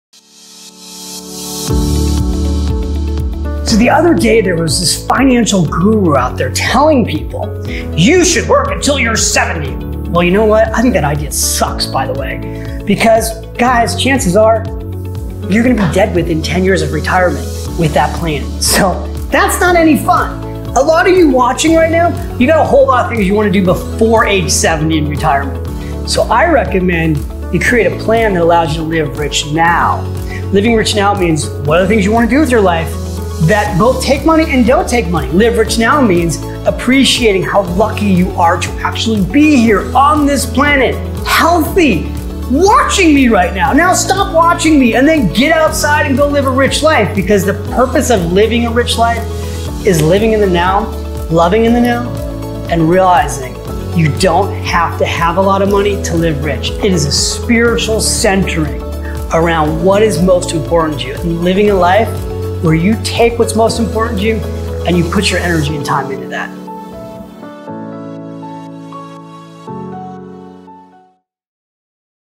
The Latte Factor | You Can Live Rich!
Working until you're 70? Doesn't sound like a great life-plan. You're already richer than you think, but it requires saying NO to the unnecessary things so that you can say YES to the things you really want in life.